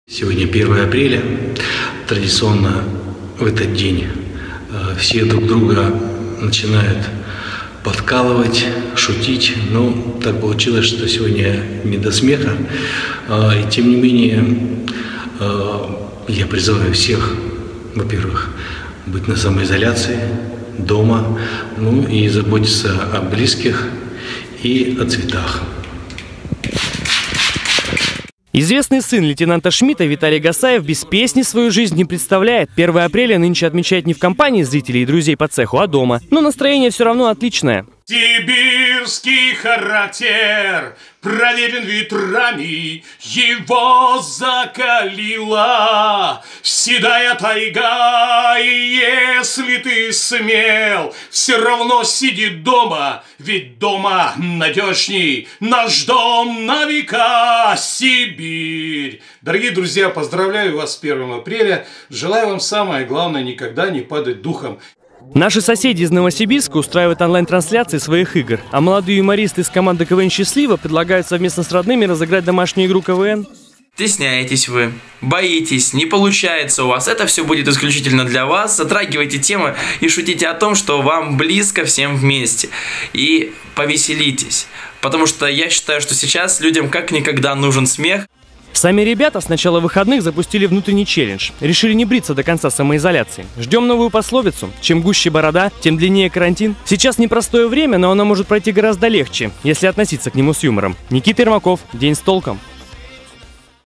День смеха: барнаульские юмористы не унывают
В период пандемии очень сложно расслабиться, тем более  веселиться. Но 1 апреля в календаре никто не отменял.  И как известно-  юмор - лекарство от всех болезней. Барнаульские веселые и находчивые, как и все находятся дома. И сегодня  своими улыбками хотят поддержать всех, кто находится на режиме самоизоляции.